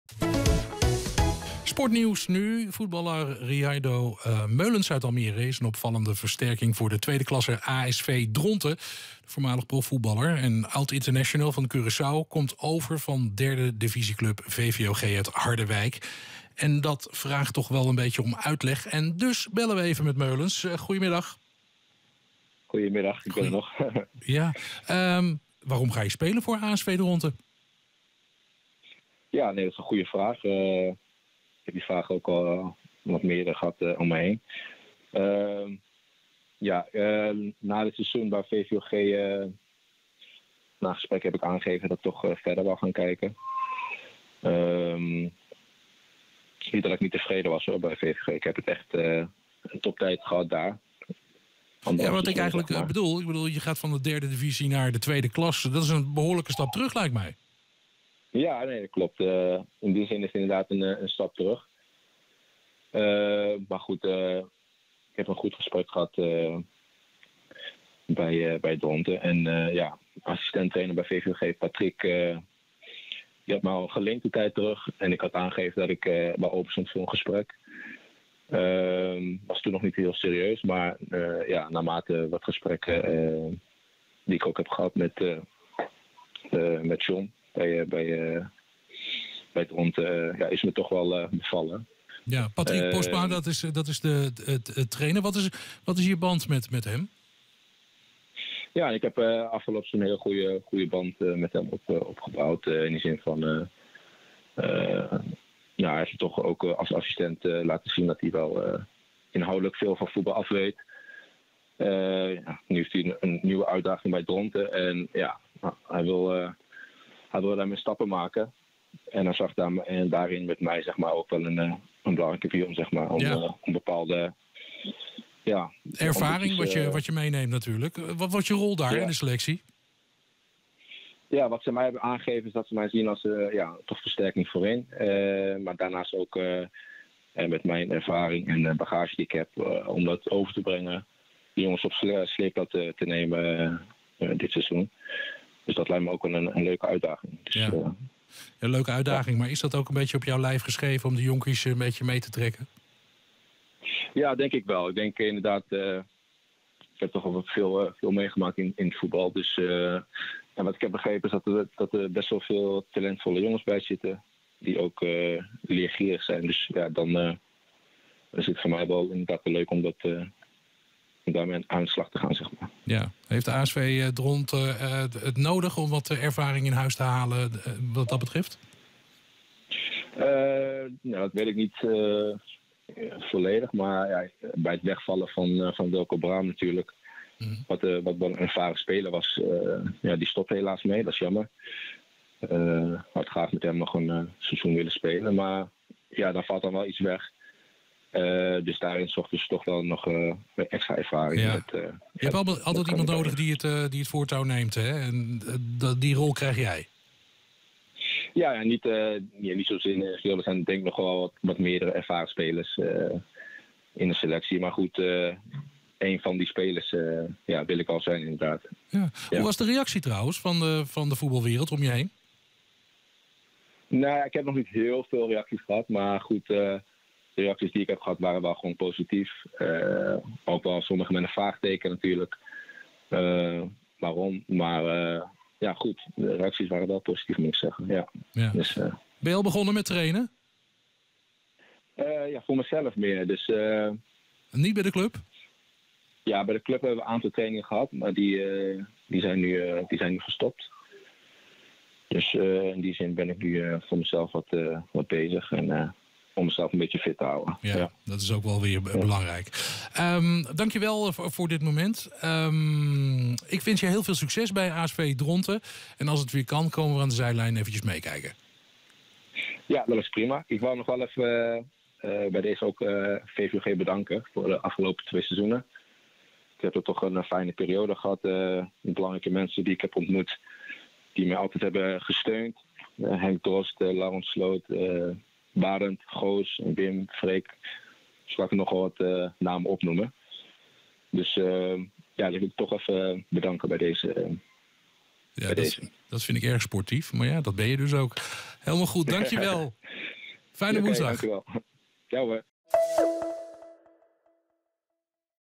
Oud-international Meulens naar ASV Dronten
Voormalig profvoetballer en oud-international van Curaçao Rihairo Meulens (32) gaat vanaf volgend seizoen spelen voor tweedeklasser ASV Dronten. Meulens speelde de afgelopen twee seizoenen voor Derde Divisionist VVOG uit Harderwijk.

Meulens speelde daarvoor voor onder meer Vitesse, AGOVV, FC Dordrecht, Roda JC, Almere City en FC Volendam. Ook speelde hij in het buitenland bij het Roemeense Rapid Boekarest en Zira FK in Azerbeidzjan.

Daarnaast speelde hij tien interlands voor het nationale elftal van Curaçao.